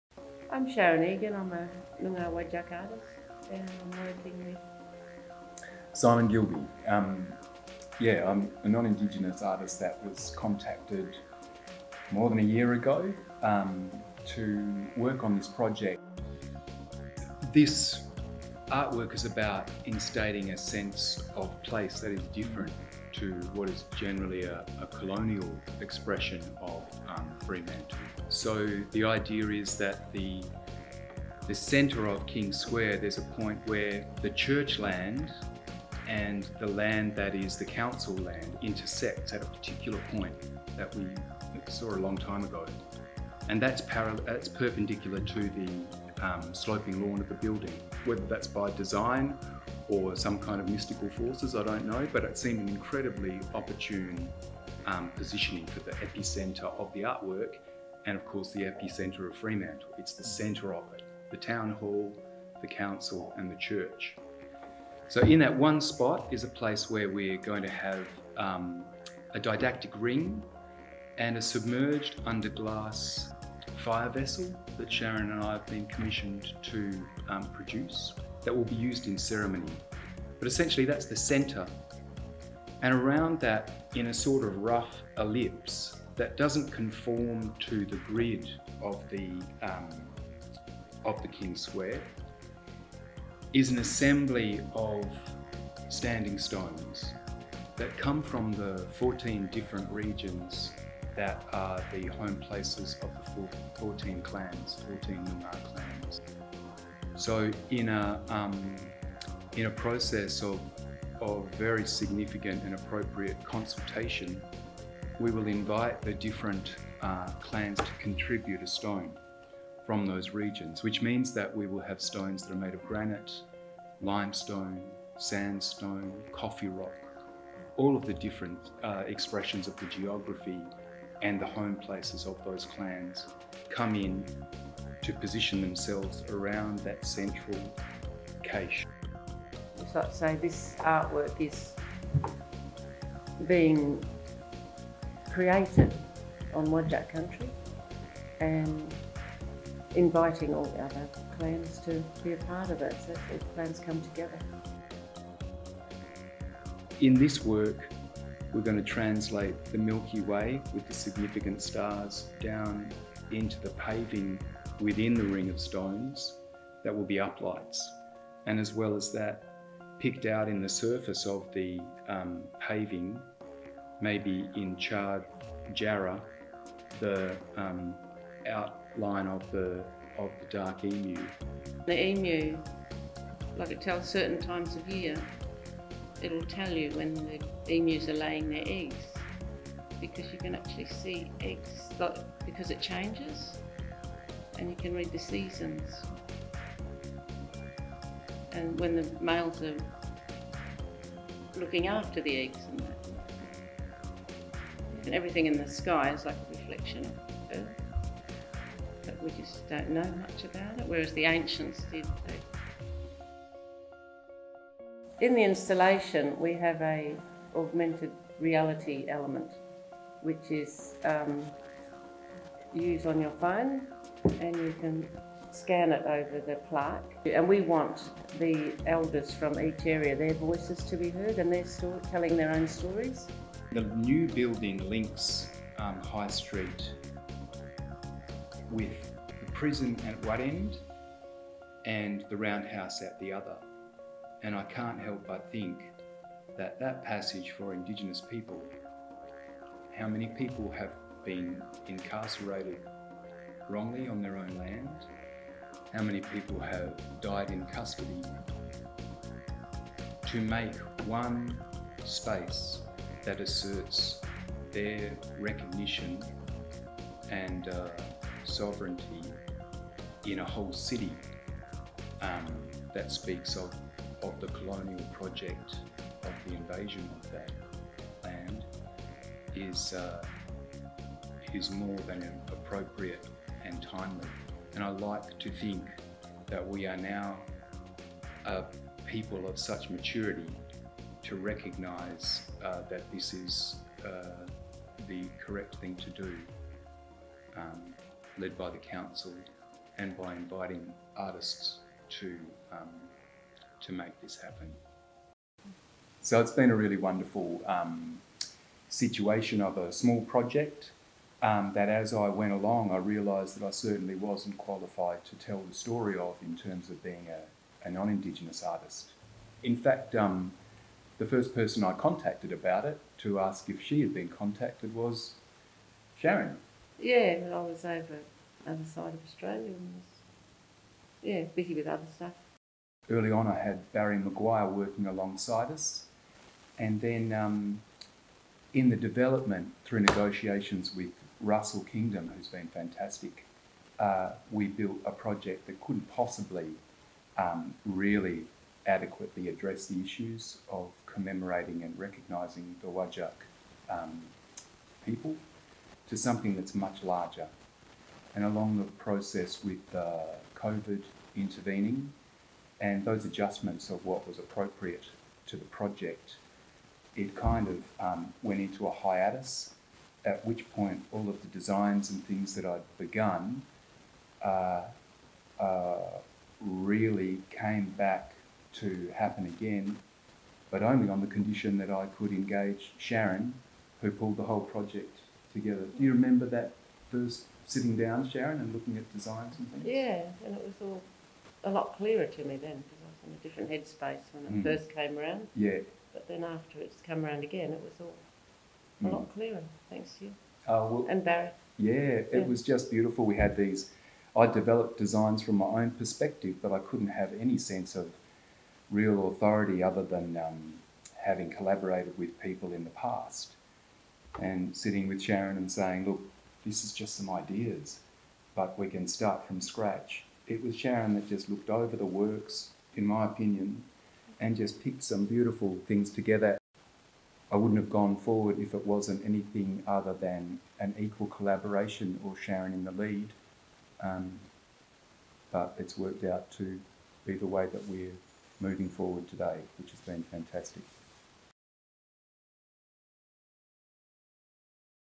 Walyalup Koort public artwork - Respect, Recognition and Reconciliation
A new public artwork could be coming to Kings Square, which interprets Fremantle's colonial past and WA's Nyoongar culture.